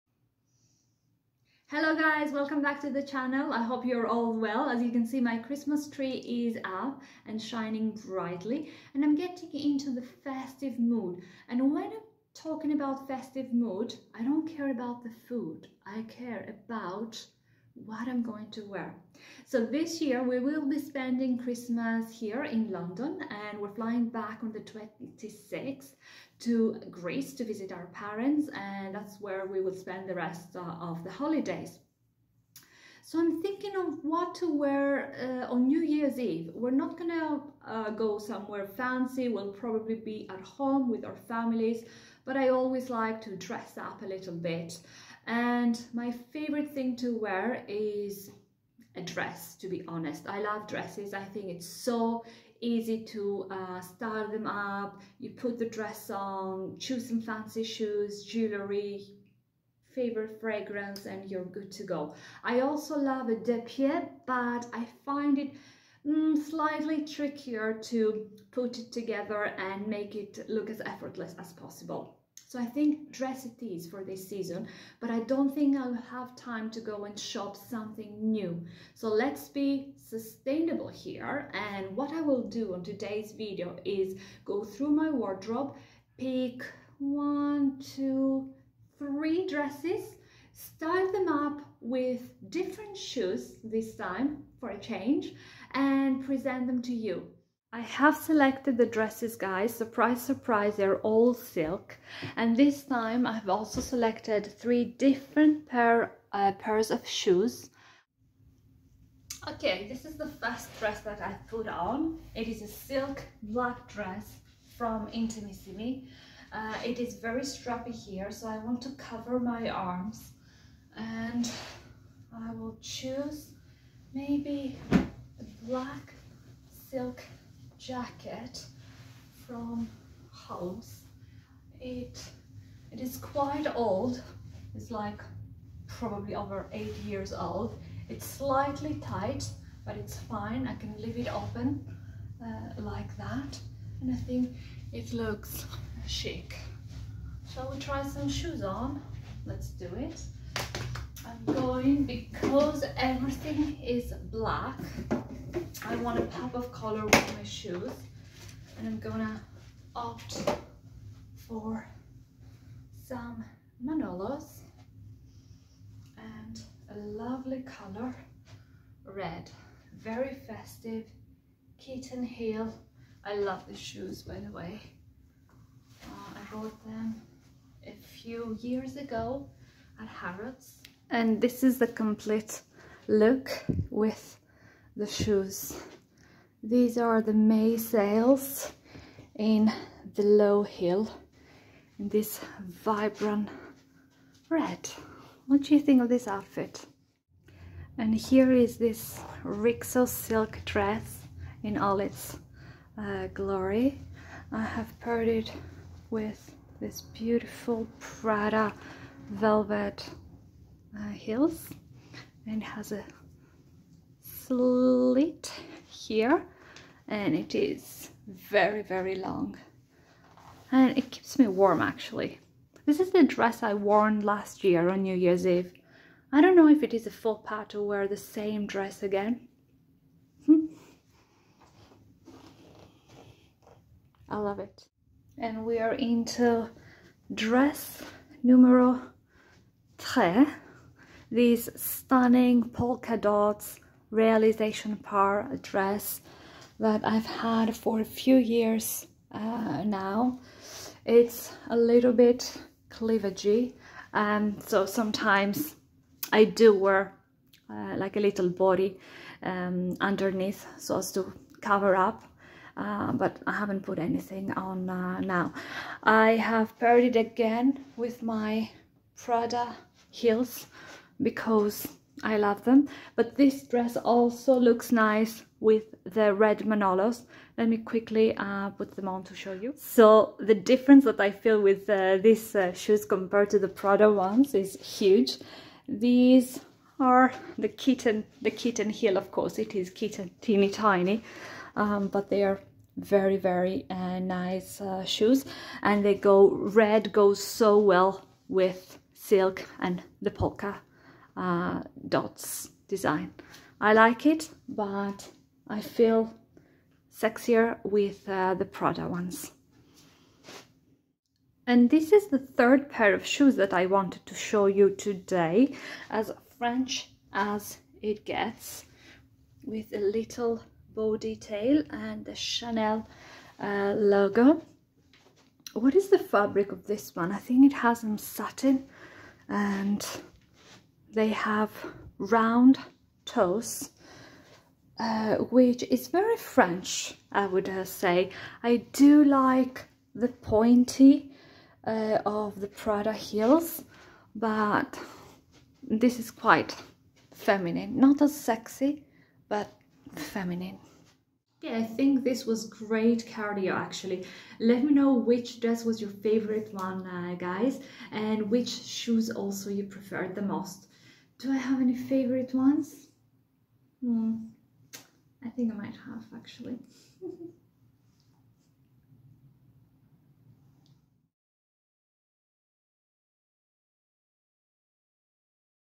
How to Style Silk Dresses for Any Occasion in 2024 | Helen Tsokana
Welcome to my latest video, where I'm shopping my wardrobe and putting together three outfits for New Year's Eve celebrations. As someone who loves maximizing my wardrobe by using key pieces that I already own, I'm excited to share these outfit ideas with you.

In this video, I've chosen three silk dresses that make me feel good and are super comfortable. I'll show you how to style each dress with shoes from Manolo Blahnik, Prada, and Chanel, creating three unique looks that are perfect for ringing in the new year in style.

I believe that you don't need to constantly buy new clothes to look great, and this video is proof that shopping your own wardrobe can be just as fun and exciting. With a little creativity and imagination, you can create stylish outfits using items you already own.

So, whether you're celebrating at home or going out with friends, you'll find plenty of inspiration in this video.

What I Wore:

Dress 1 from Intimissimi
Dress 2 from Rixo
Dress 3 from Realisation Par
Shoes from Manolo Blahnik, Prada, and Chanel

Helen 💋